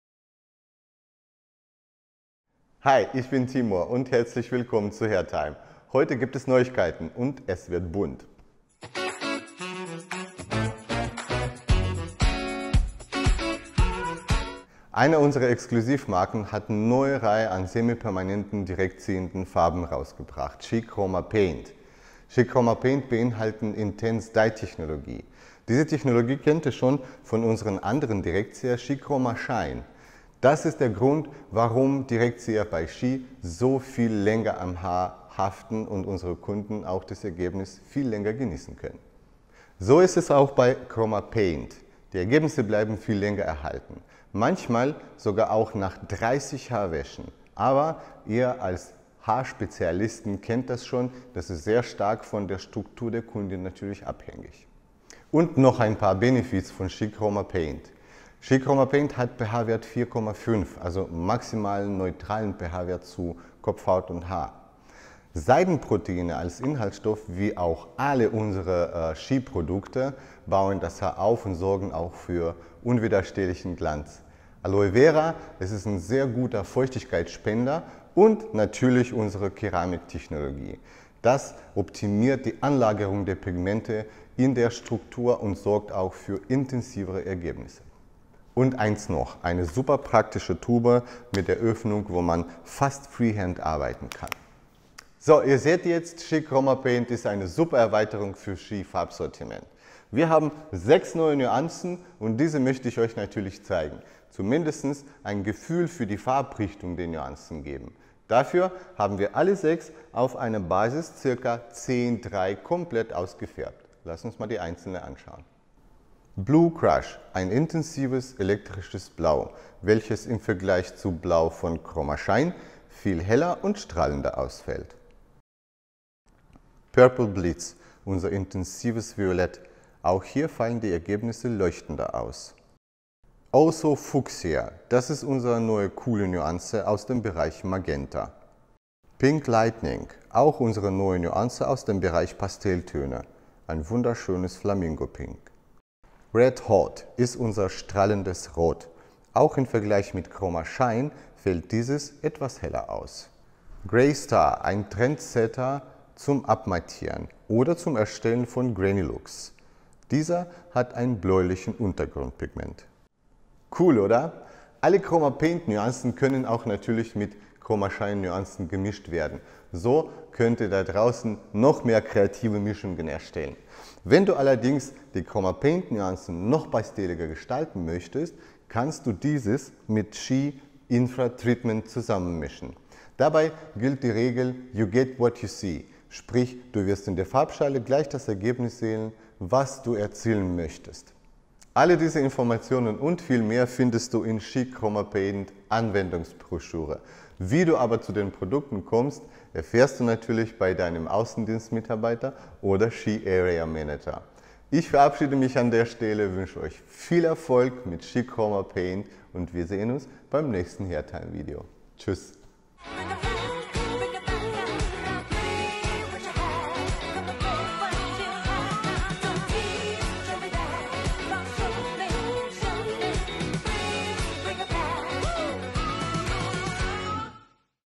Direktziehende Farben von CHI – Chroma Paint
Es wird bunt mit CHI Chroma Paint! 🌈 Timur zeigt euch heute in unserem hairtime Video die neuen direktziehenden Farben von CHI.
Welche Inhaltsstoffe beinhalten die Produkte? Wie lange hält das Ergebnis? Und wie viele Nuancen gibt es zur Auswahl? Schaut rein und überzeugt euch selbst! 😊